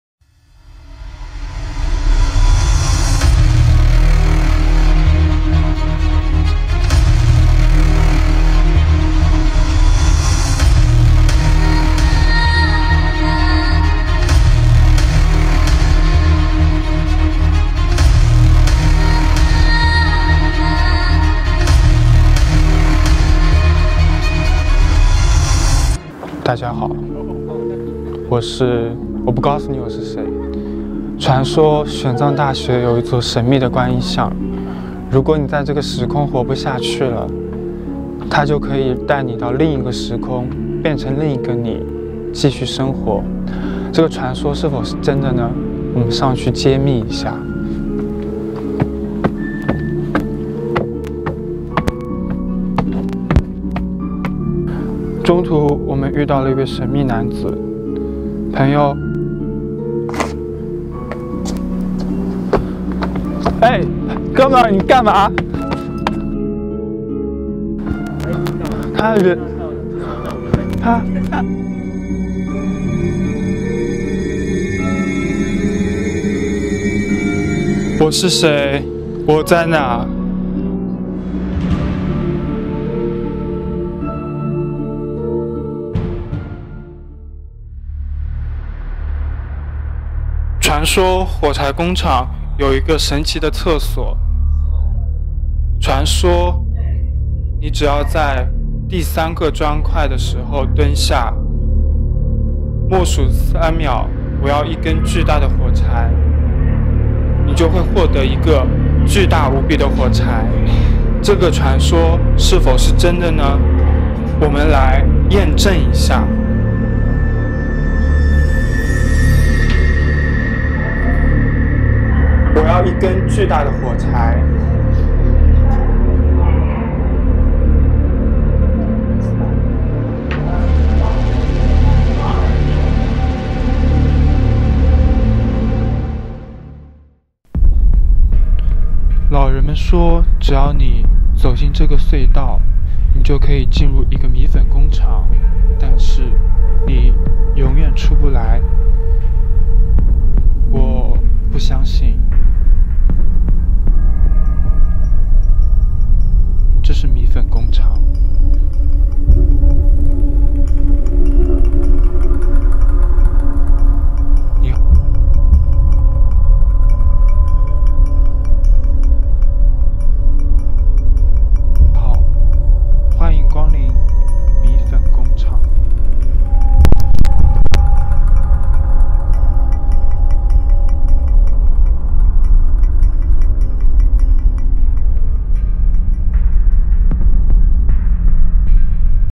許文傑 翁野峻三分鐘
浙江越秀外國語學院 網路傳播學院的同學
來玄奘大學做一個星期的移地教學
主題是:短視頻的製作